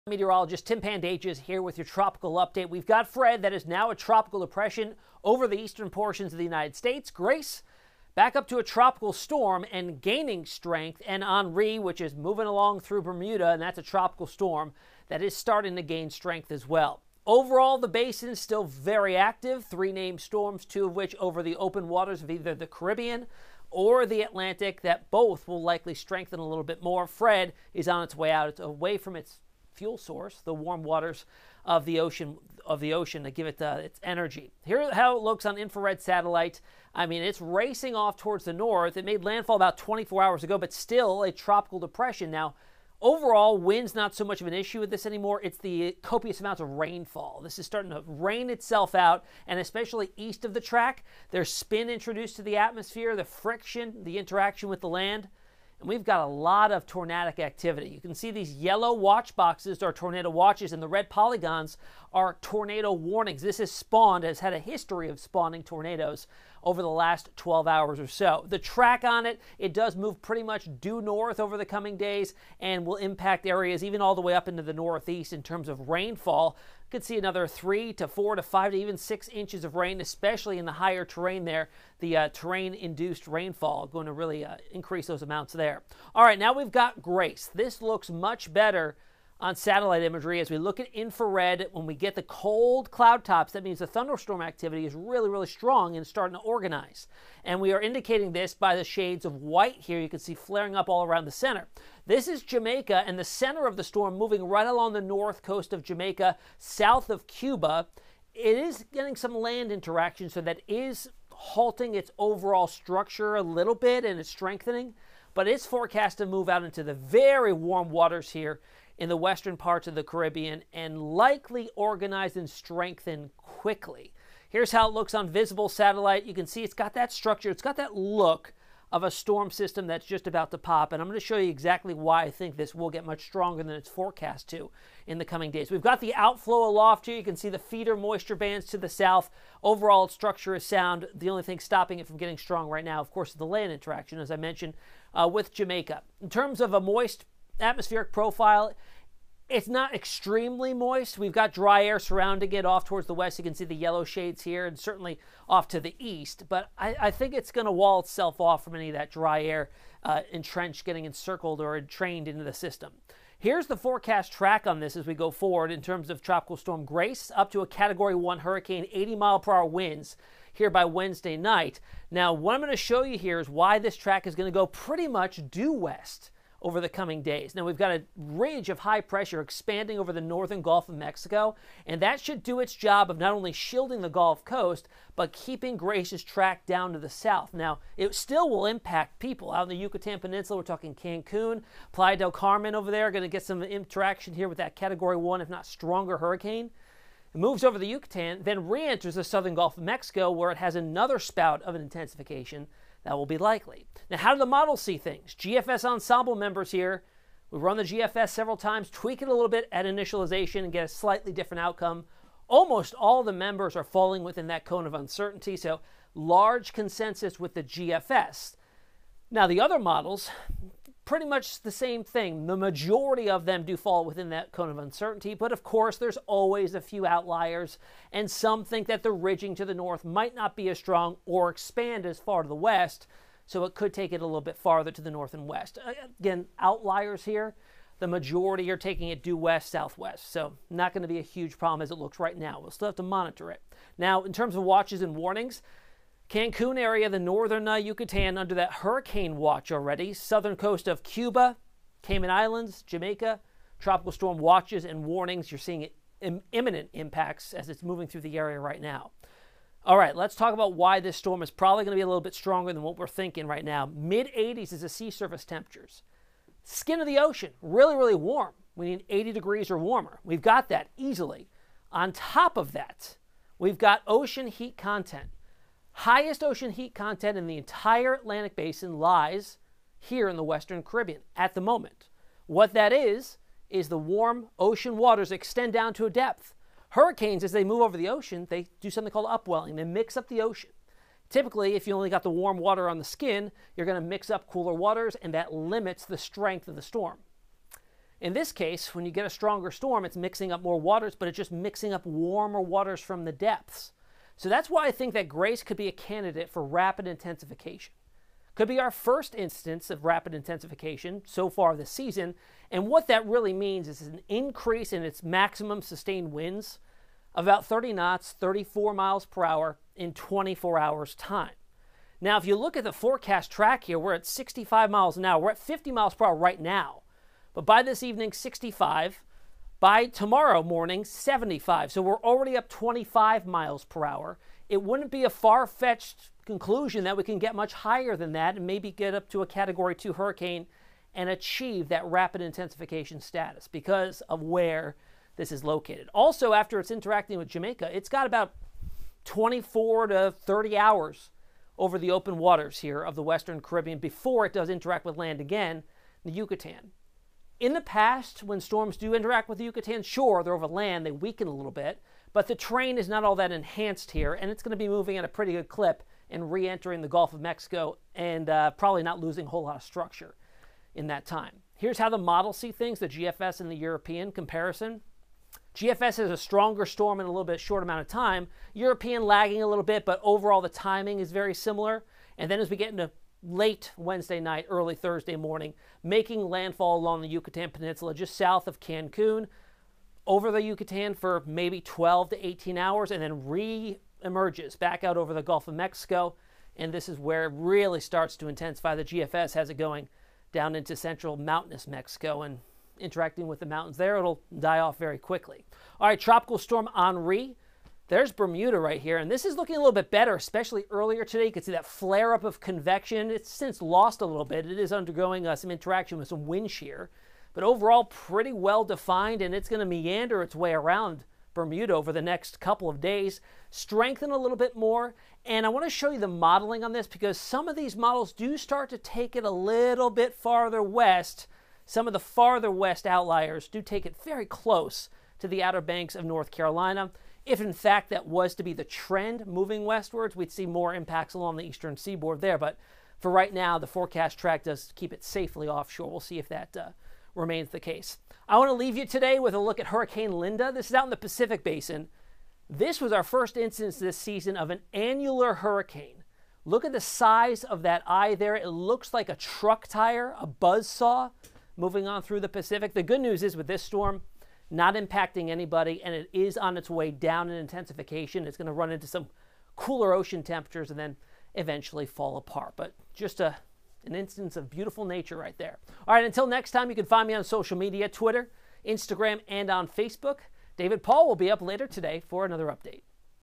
In the tropics: Tropical Storm Grace, TD Fred and TS Henri
KHOU 11 Meteorologist Tim Pandajis is tracking three tropical systems.

KHOU.com/hurricane